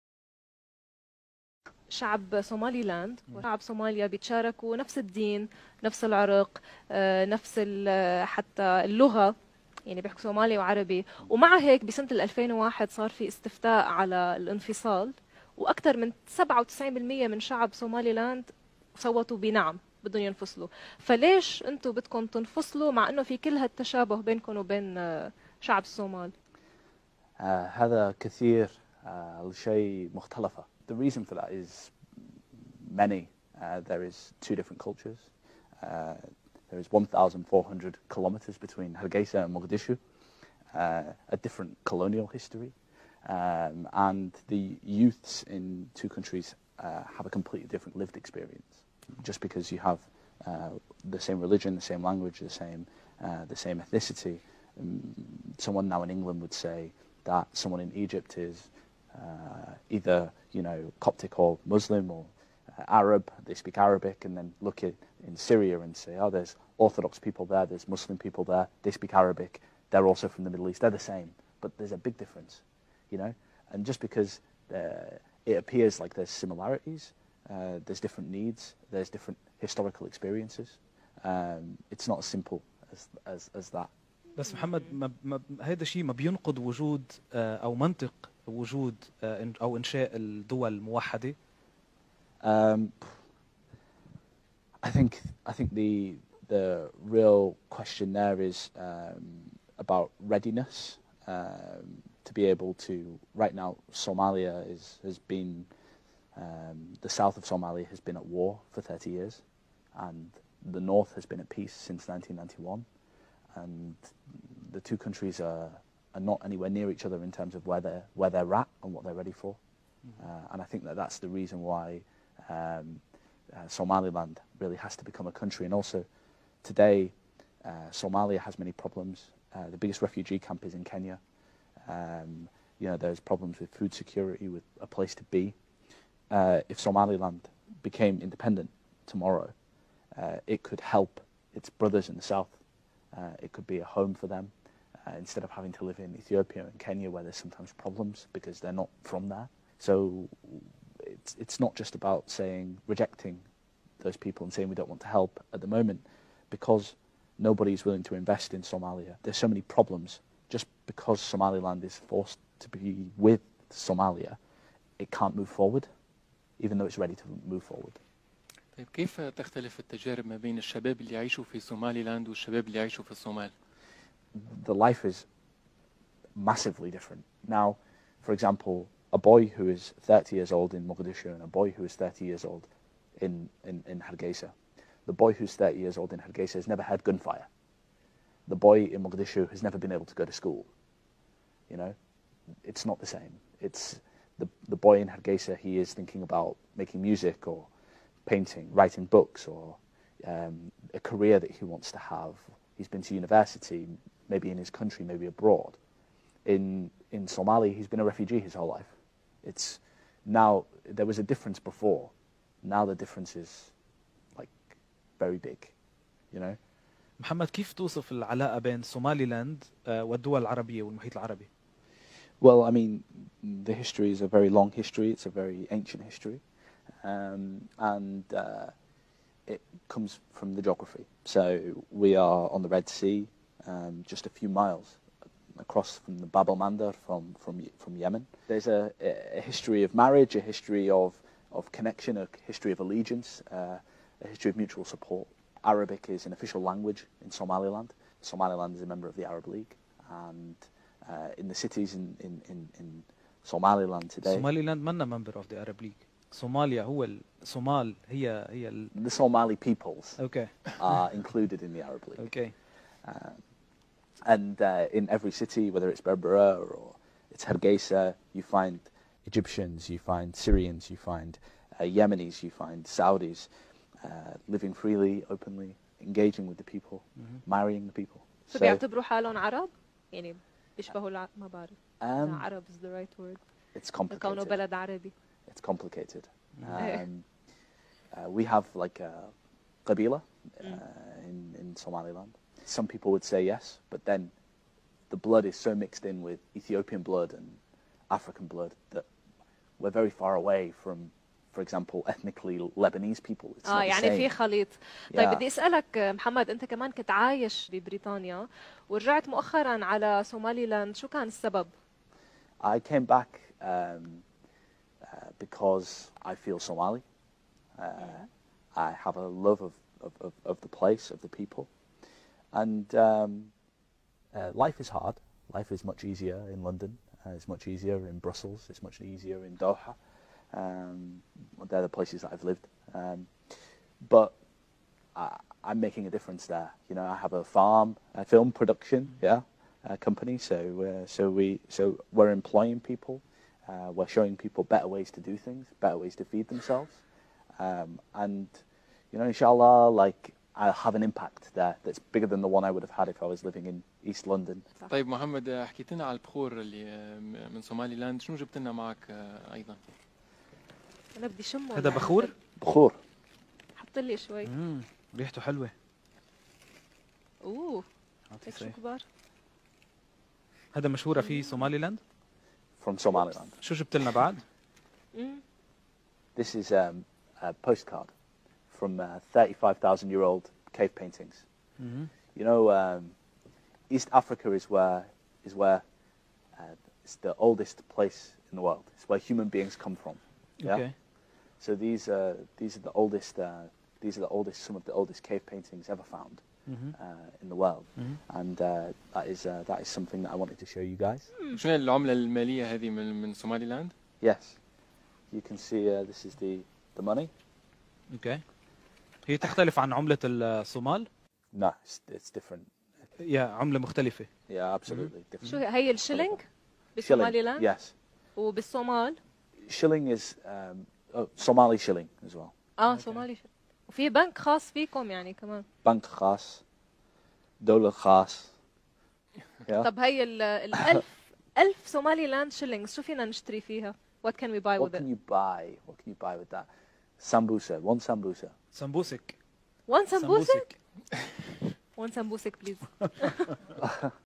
A young Somalilander man has explained the difference between Somaliland and Somalia BRILLIANTLY.
A young Somalilander man has explained the difference between Somaliland and Somalia in a way that is amazing. inspirational